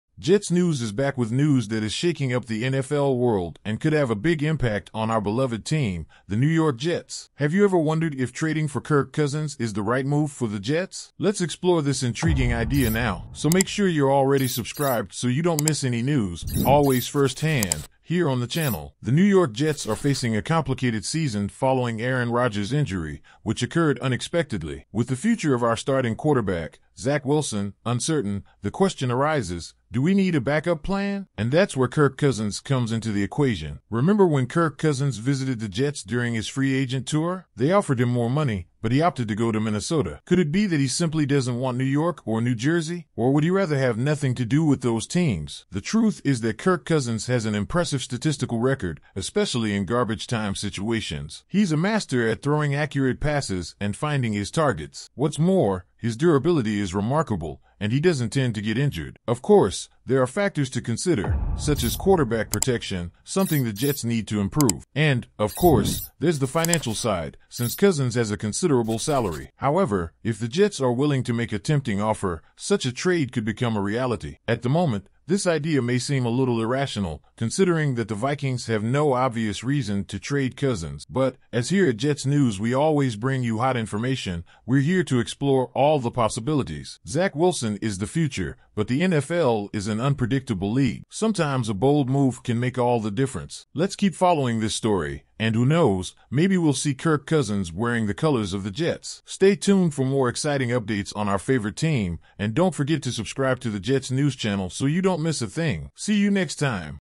🔥URGENT! JETS' SURPRISE MOVE: TRADE FOR KIRK COUSINS!? NEW YORK JETS NEWS!
_*🛫NEW YORK JETS NEWS!🆕*_

*Summary*
A surprising turn of events is stirring the NFL and the hearts of Jets fans. With the uncertainty surrounding Zach Wilson's future and Aaron Rodgers' injury, the question arises: could Kirk Cousins be the answer for the Jets? Find out why this idea is gaining momentum and how it could change the fate of our beloved team. 🏈🔥